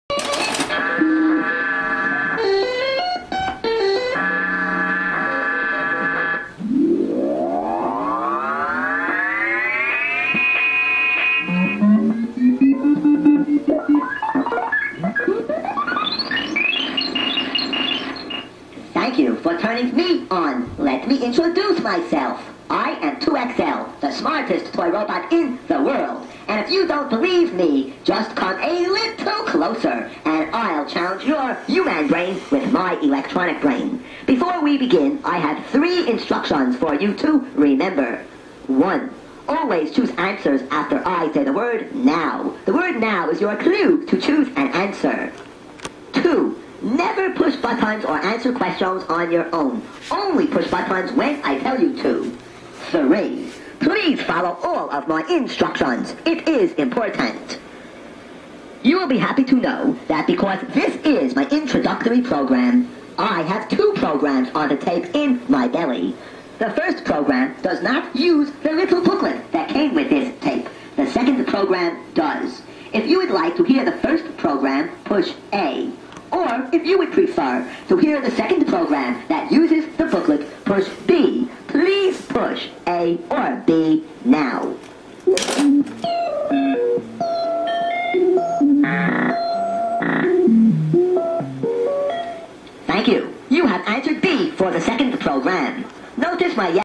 2XL Robot - Introducing himself
Here is a nifty little guy that I picked up at an estate auction. He seems to work flawlessly! He is on Ebay under the seller name jaime729 starting 1/1/12 and ending 1/7/12.